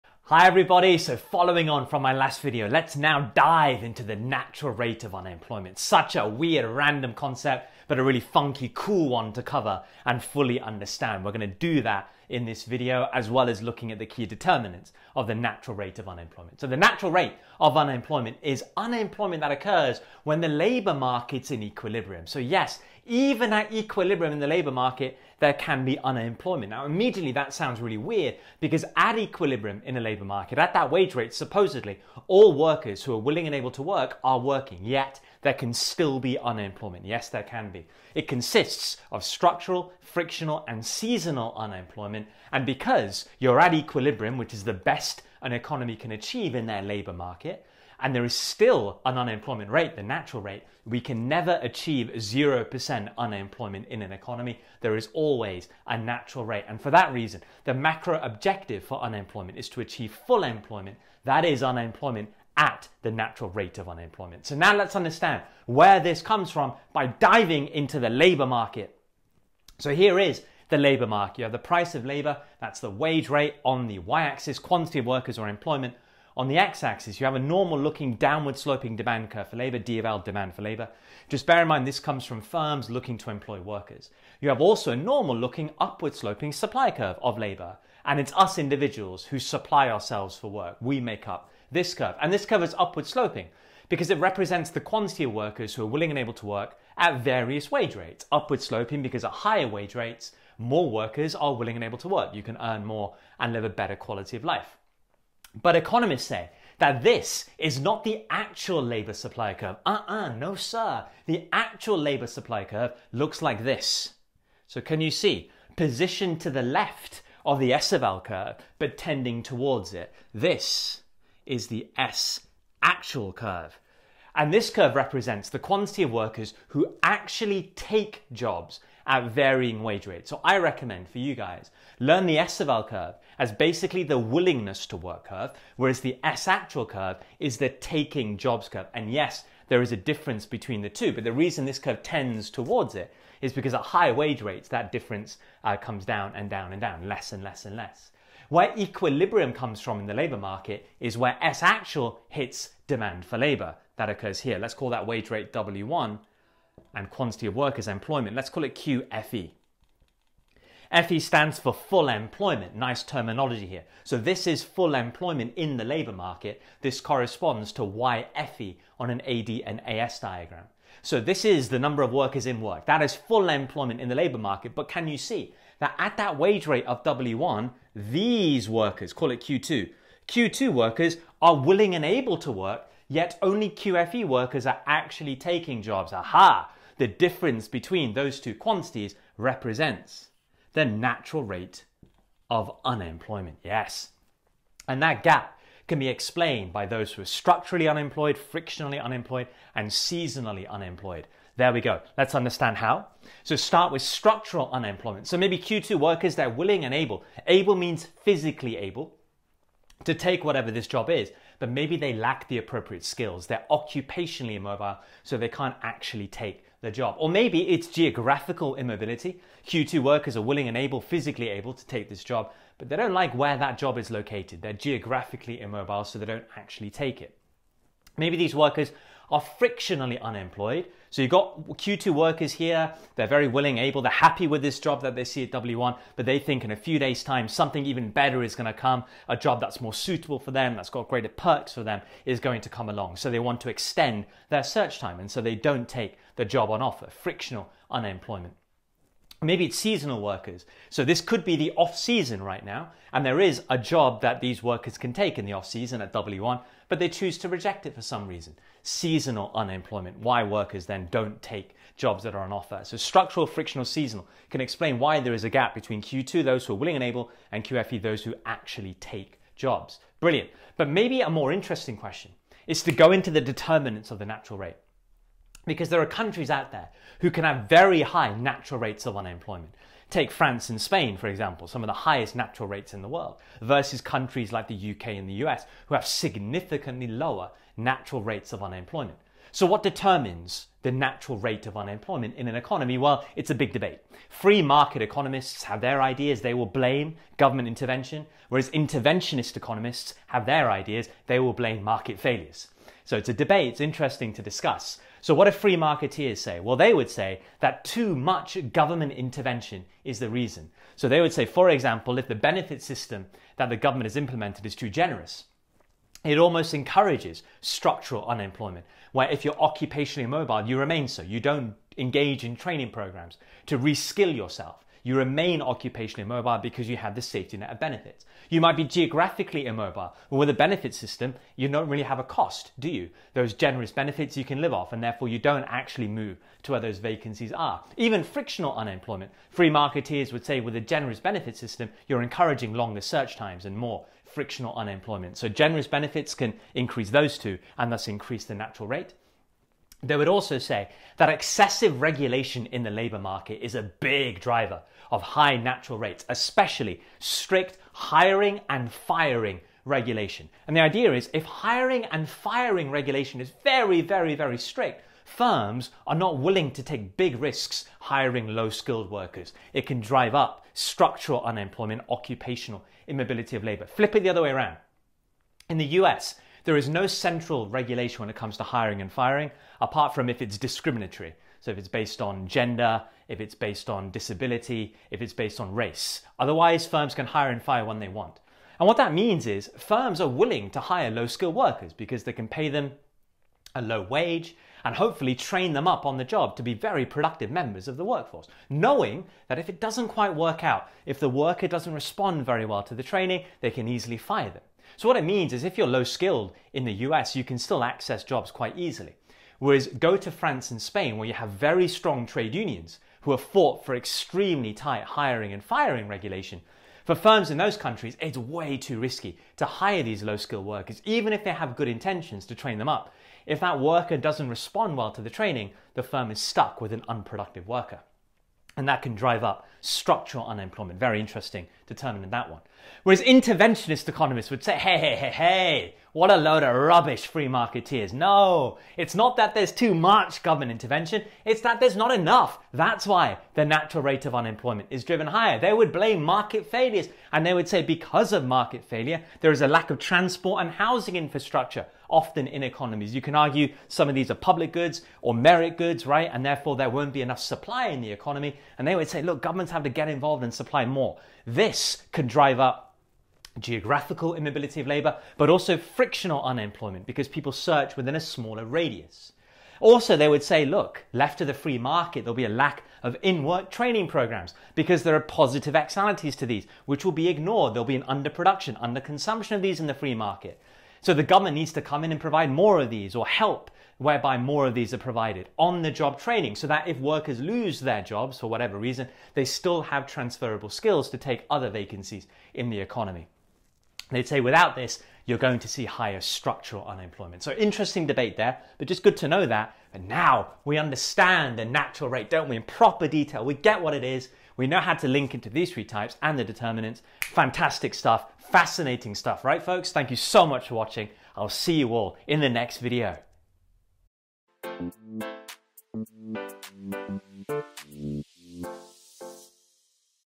Y1 22) The Natural Rate of Unemployment
The Natural Rate of Unemployment. Video covering the natural rate of unemployment; the types of unemployment within the natural rate and the causes of the natural rate of unemployment